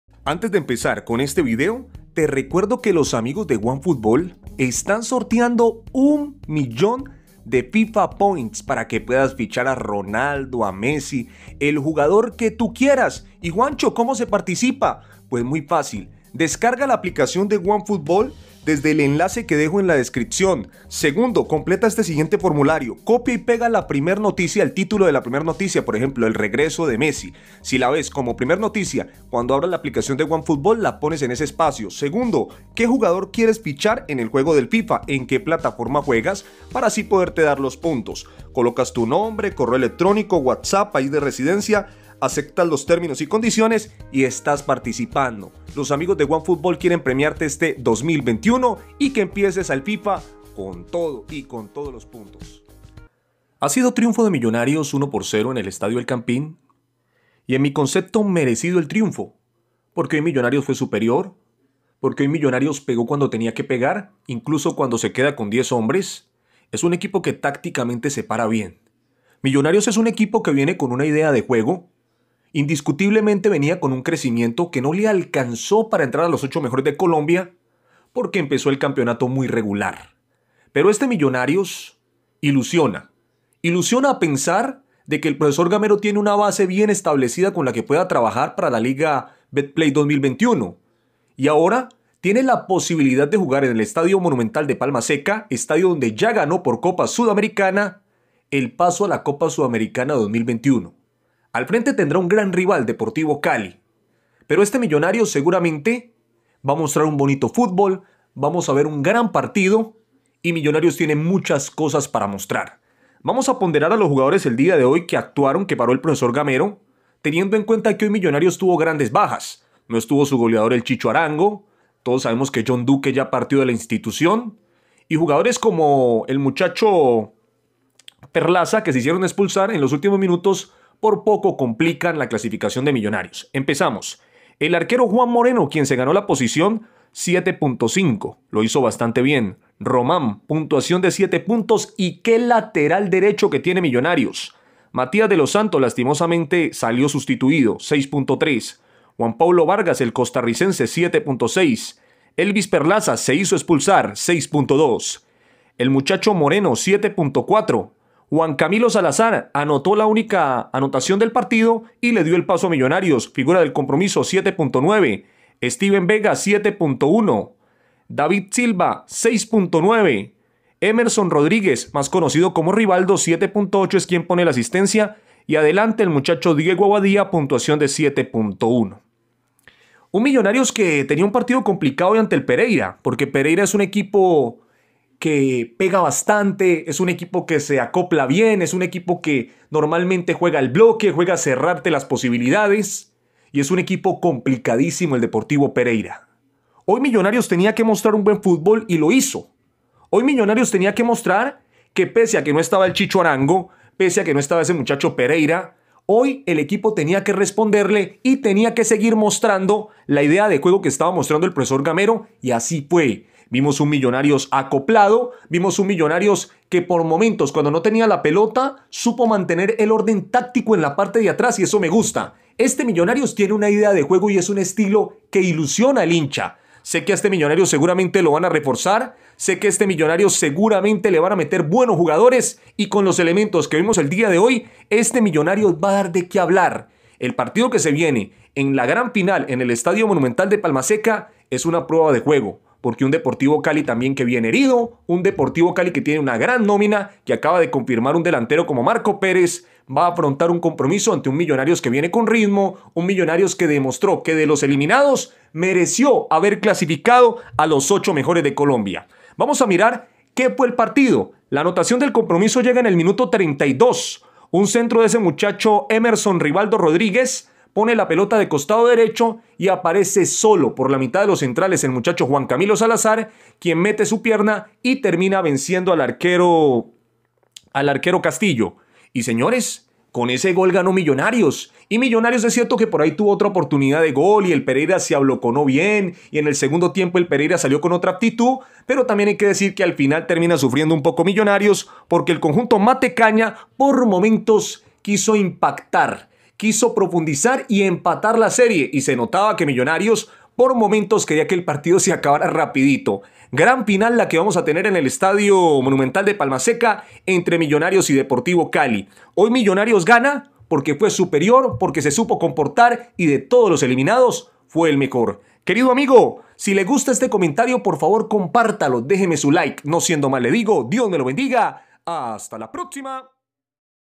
Millonarios 1 - 0 Pereira 💣 los embajadores ganan la liguilla 💙 visitaran al Cali por cupo a copa.
Link de Descarga OneFootball ☝🏻

Todas las noticias de actualidad futbolísticas de día de hoy.

No te pierdas a segunda hora las noticias del futbol internacional por este canal.